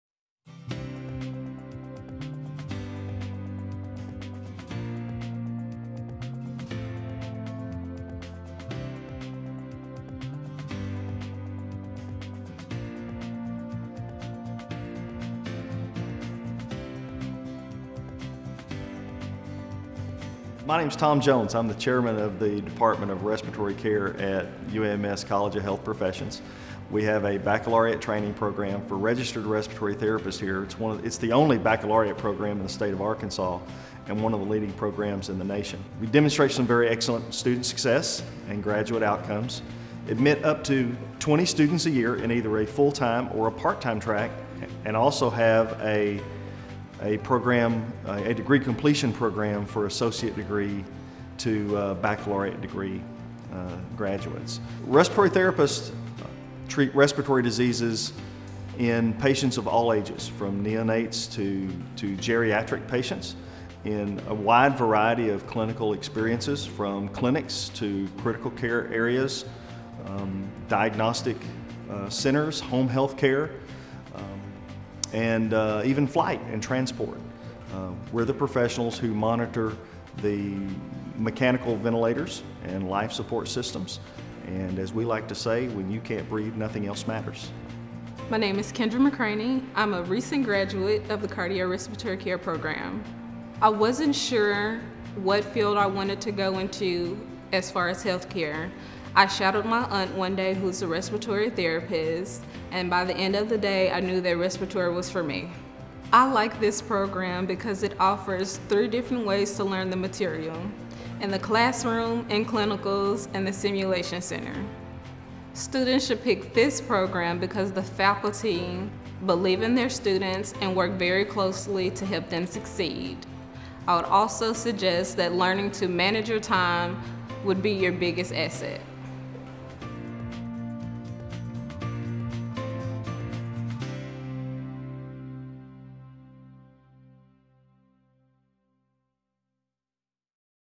UAMS College of Health Professions — Bachelor of Science in Respiratory Care Program
Explore a career as a Respiratory Therapist. The UAMS College of Health Professions offers the only Bachelor of Science degree in Respiratory Care in Arkansas. for more details.